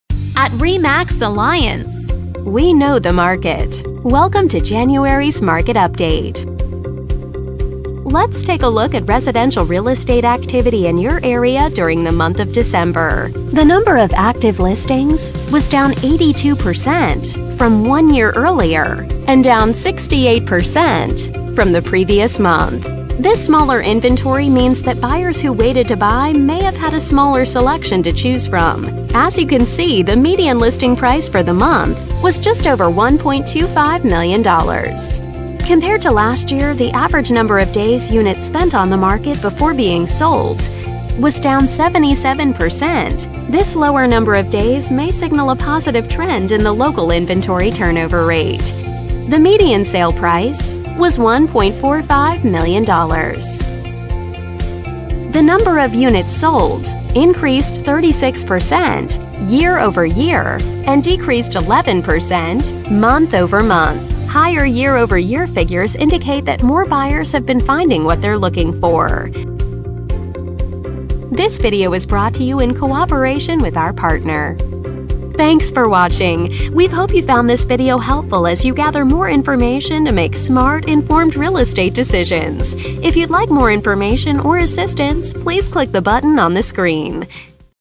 Greenwood Village,CO, Real Estate Market Update from REMAX Alliance,January, 2021
REMAX Alliance Video Update recapping market activity within Greenwood Village, CO, during the month of January, 2021